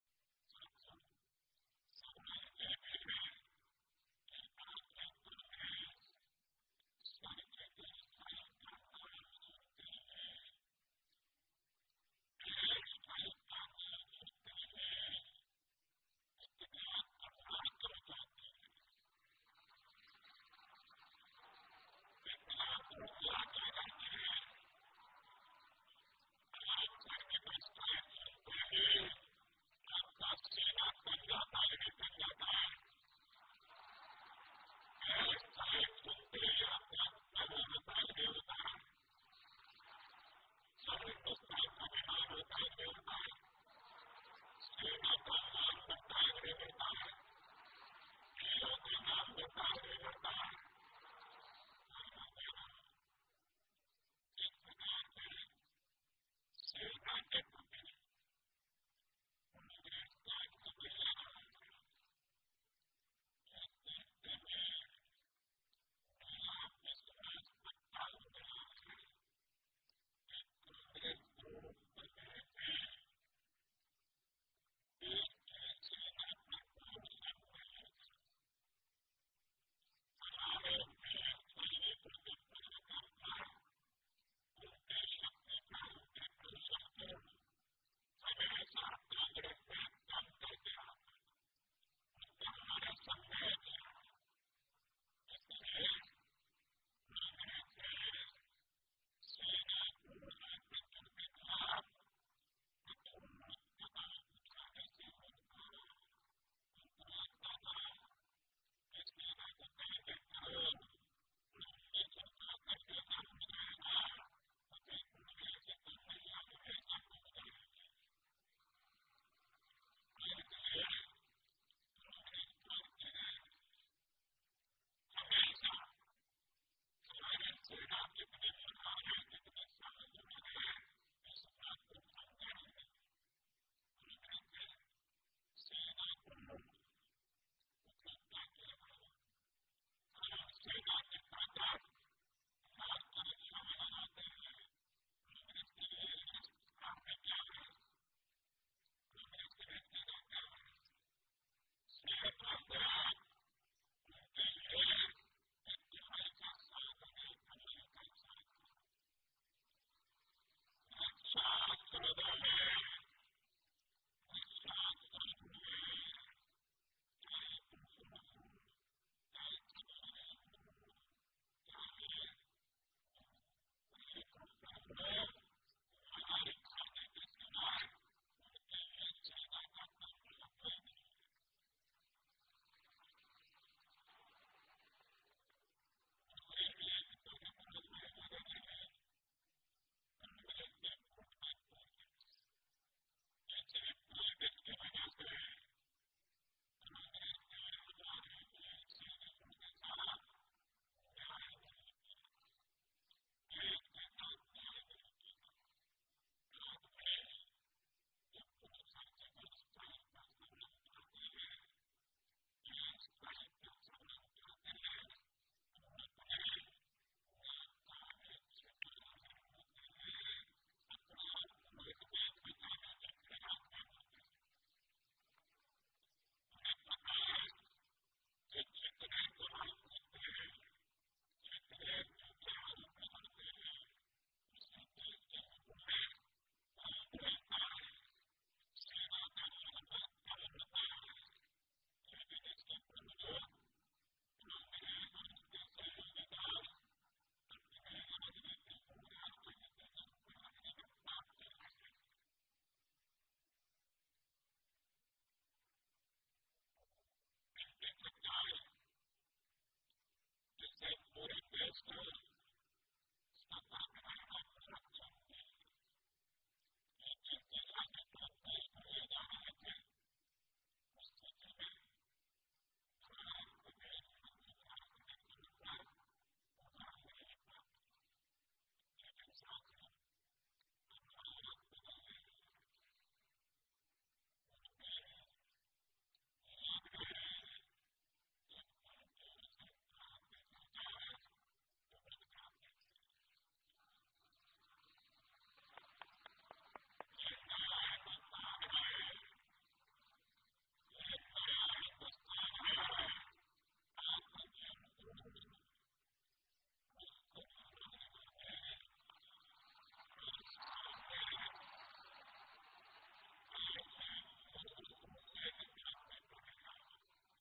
ये नया हिंदुस्तान है, आतंकियों को घुसकर मारेगा भी और दुनिया के सामने बेनकाब भी करेगा: PM
Prime Minister Narendra Modi addresses Public Meeting at Kathua, Jammu & Kashmir